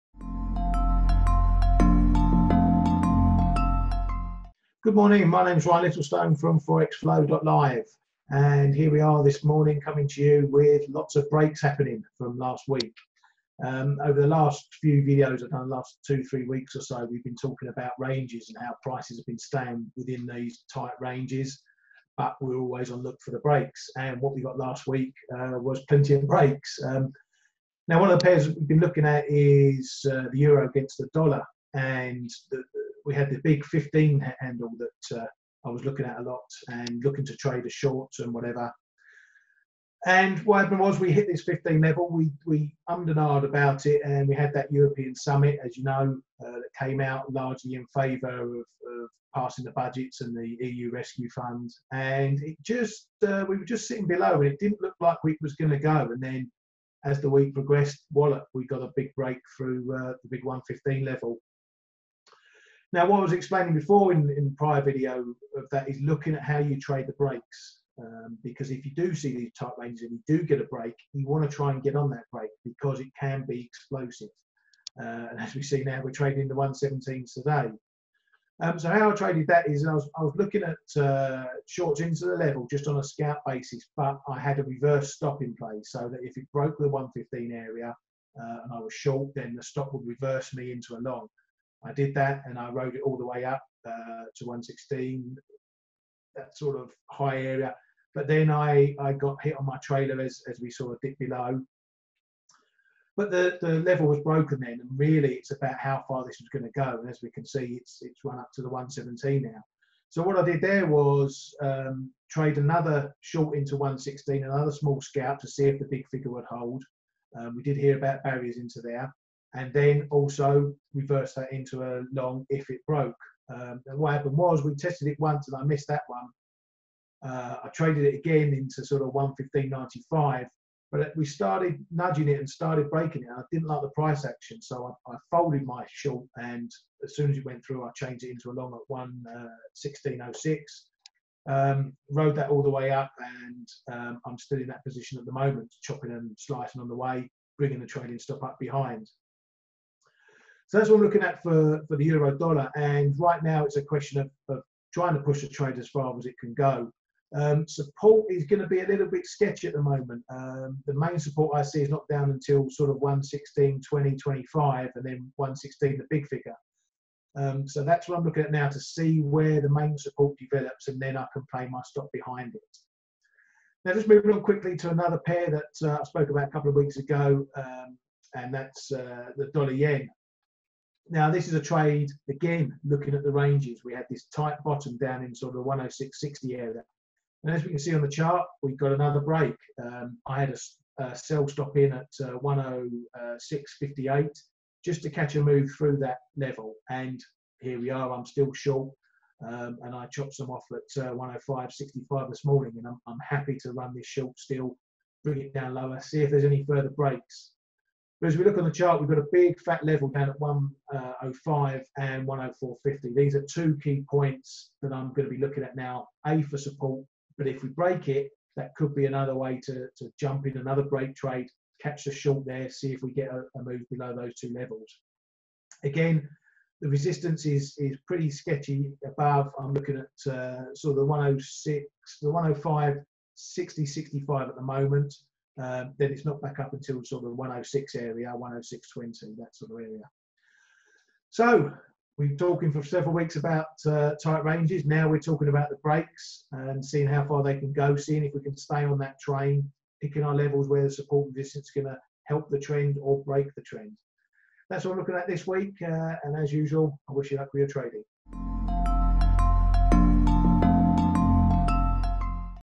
Euro and Yen
In what has been an interesting week for the Euro, Ryan reflects on his calls from last week, and whether he made the right trades. He also takes another look at the USDJPY pair to see if there's any interesting action coming up.